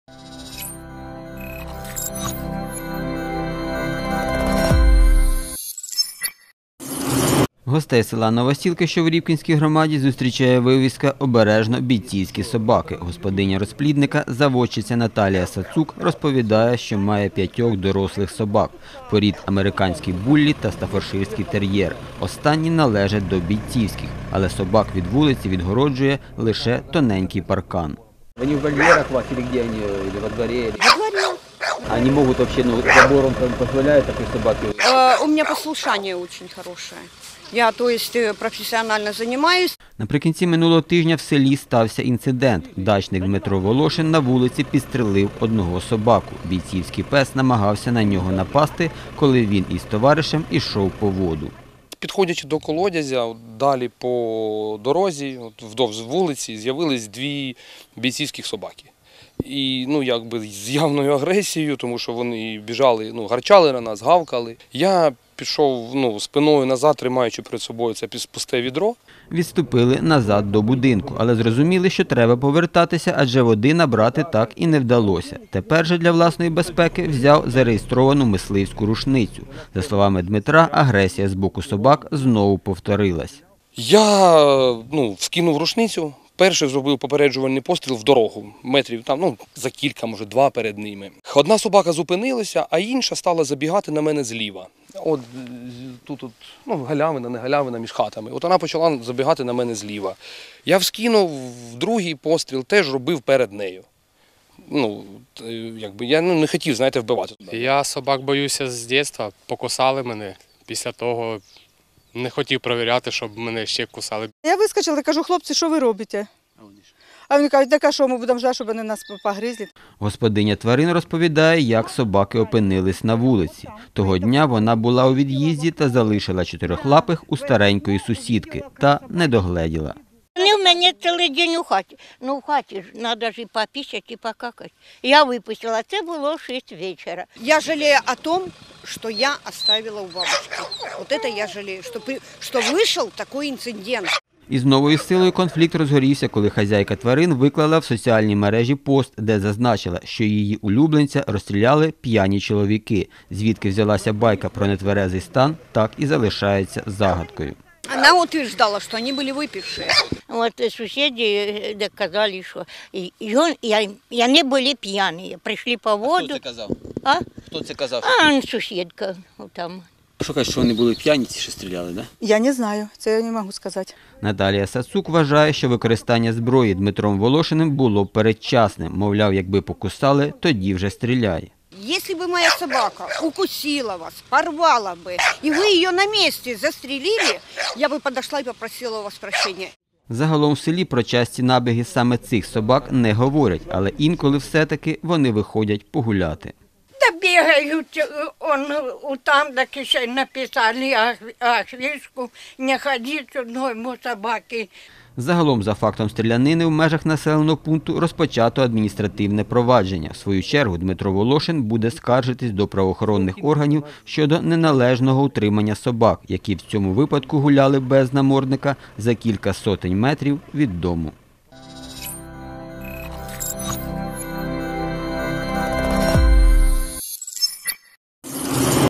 Незаконный «бойцовский» питомник или «пьяный» стрелок: собачий скандал на Черниговщине
На Черниговщине пес бойцовской породы напал на людей. Бывший участник боевых действий был вынужден защищаться стрельбой.